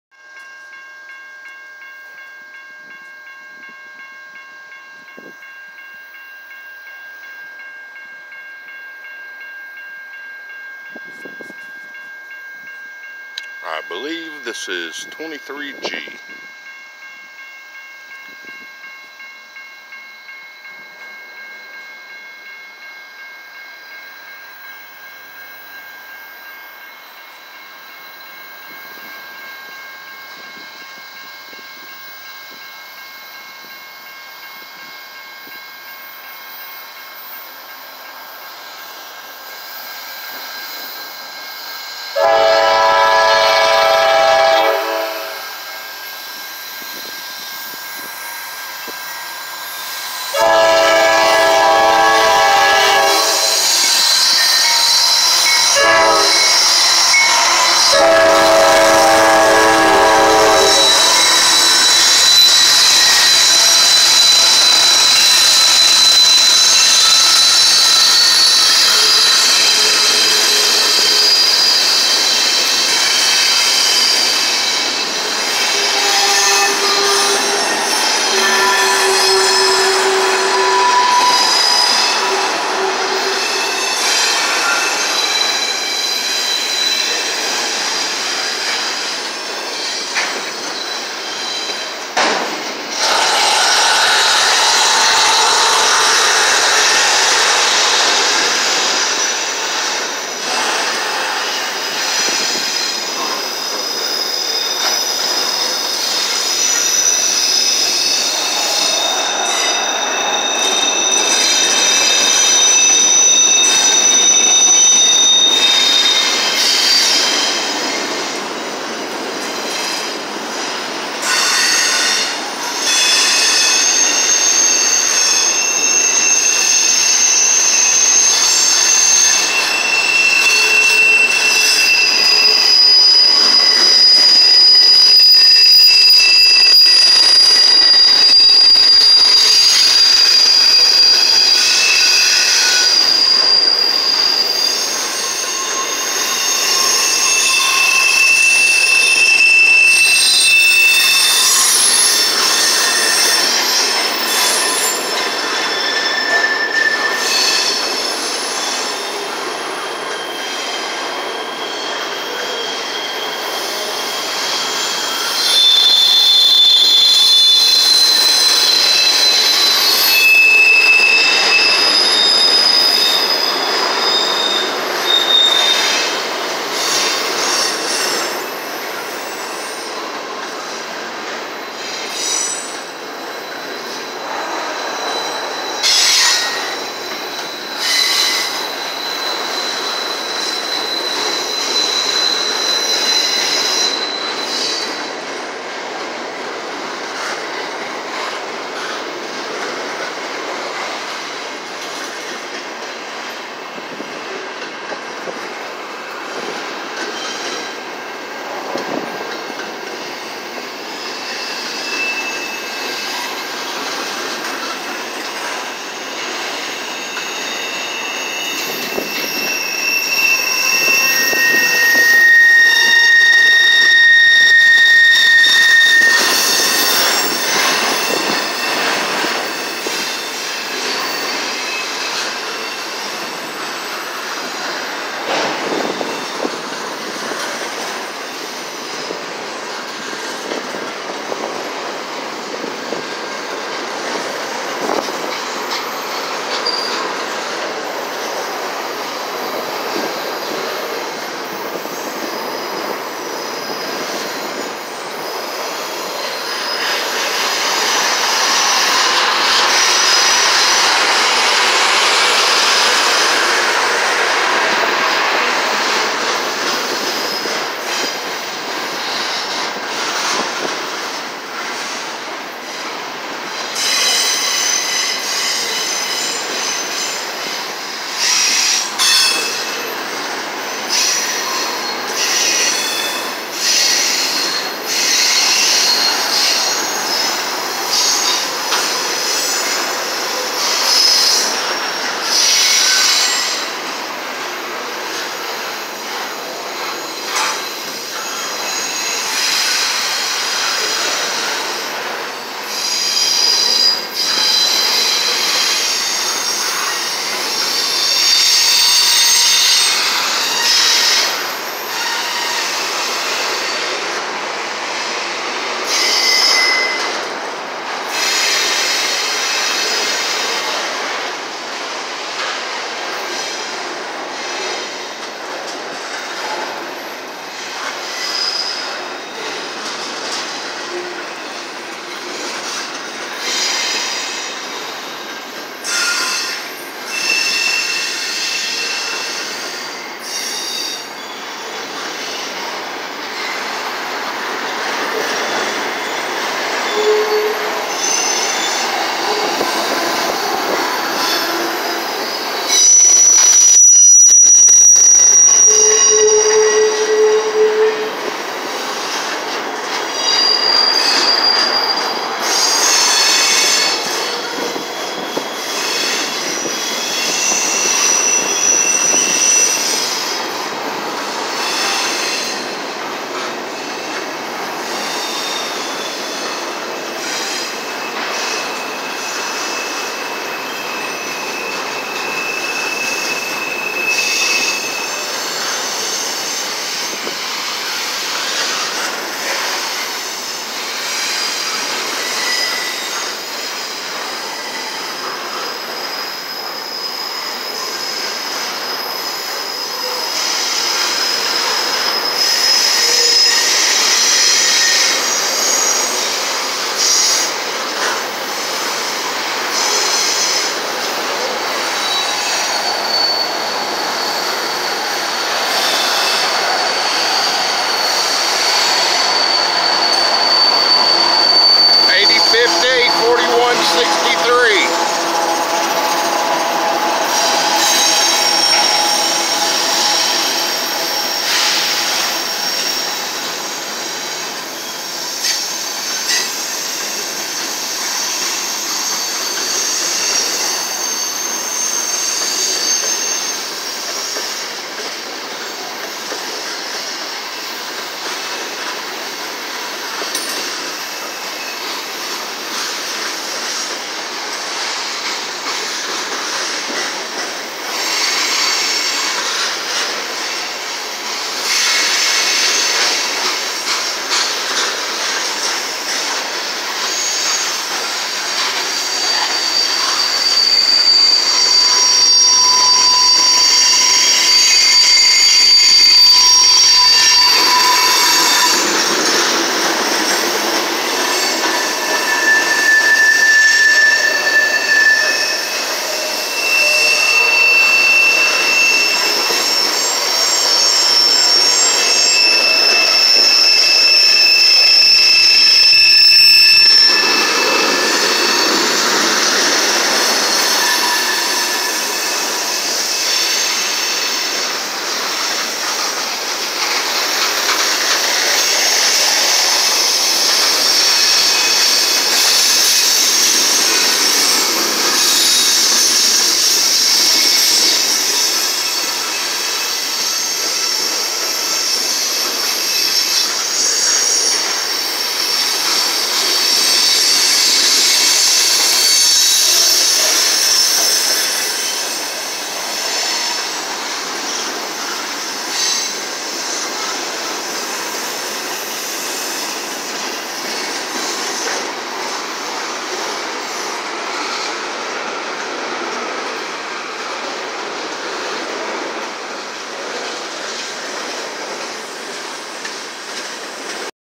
23g with NS 4136, NS 9973, Dpus after stacks NS 8050 & NS 4163 Clinton TN 6sep20
23g NS 4136, NS 9973, dpus NS 8050 & NS 4163